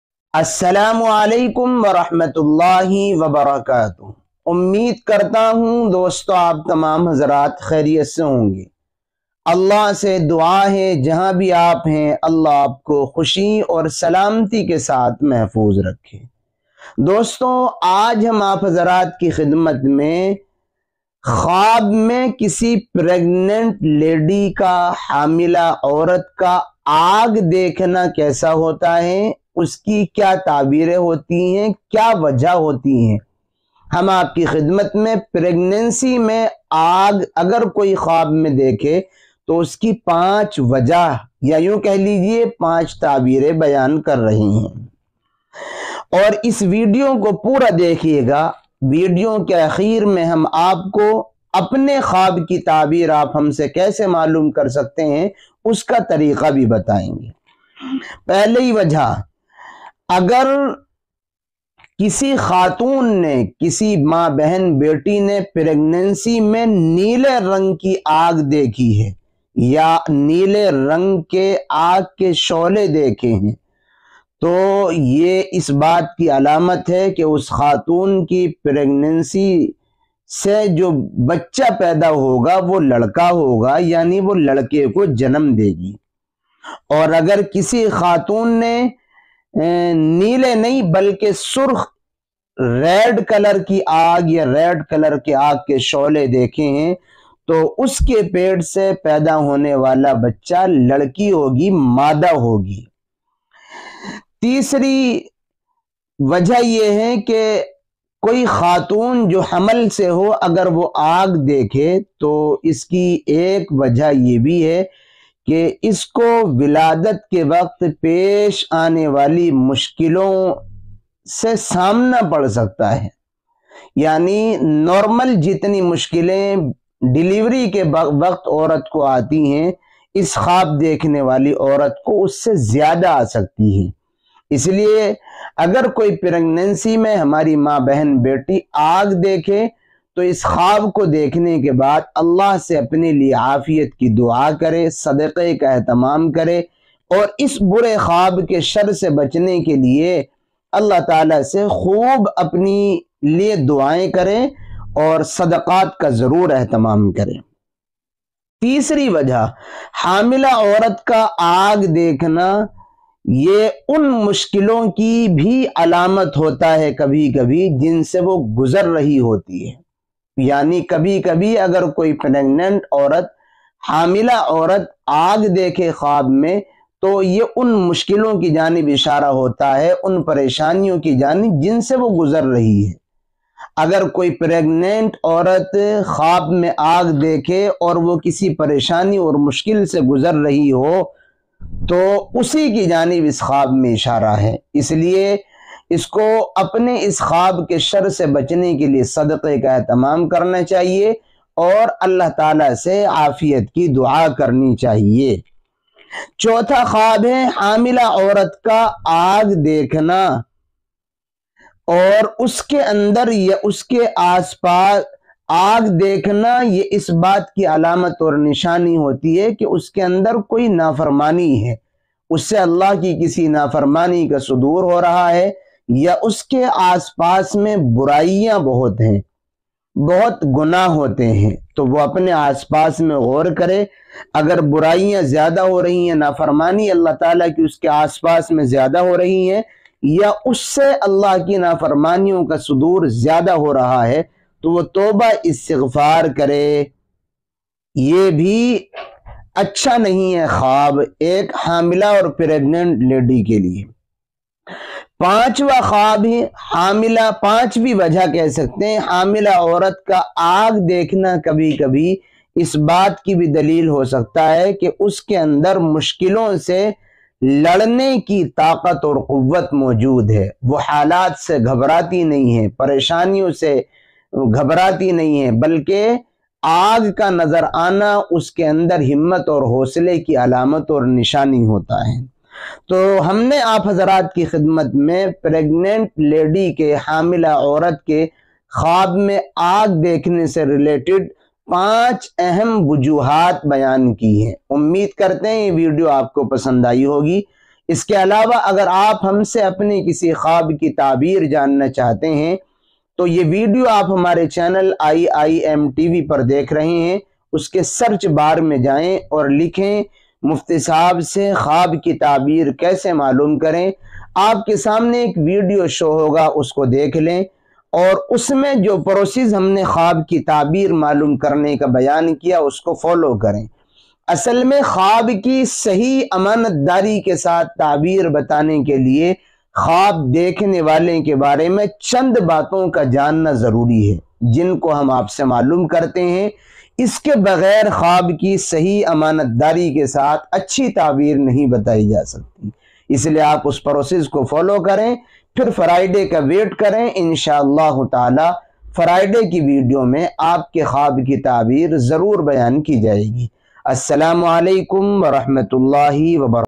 Khwab Mein Aag Dekhna ll Khwab Ki Tabeer ll Sapne Mein Aag Dekhna ll IIMTv ll Mufti Javed Ameeri
Dosto Hamne is video mein Khwab me aag dekhne ki Tabeer bayan ki hai video achchi Lage to dosto ko share kare!